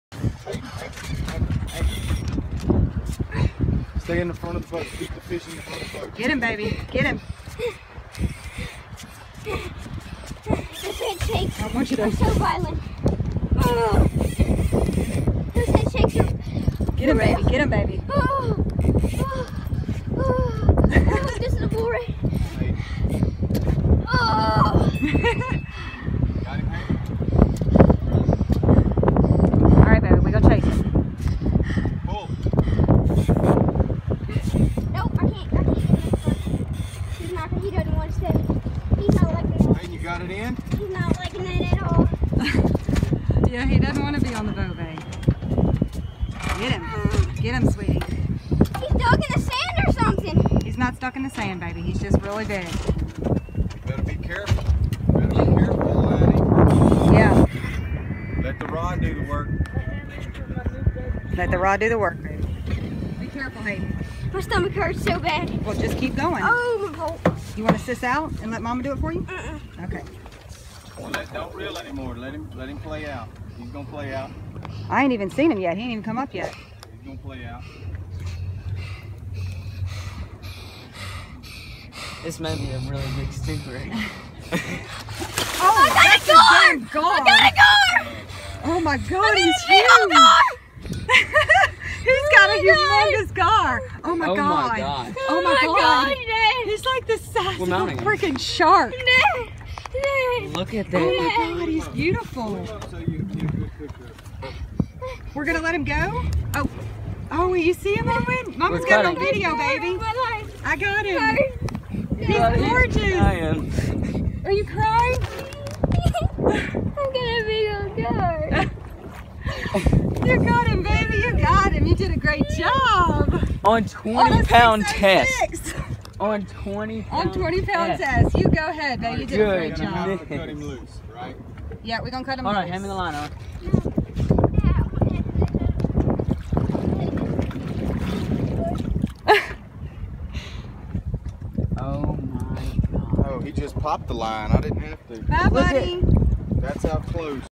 Big Gar caught by my son
My 9 year old son reeling in Gar Grand Isle, LA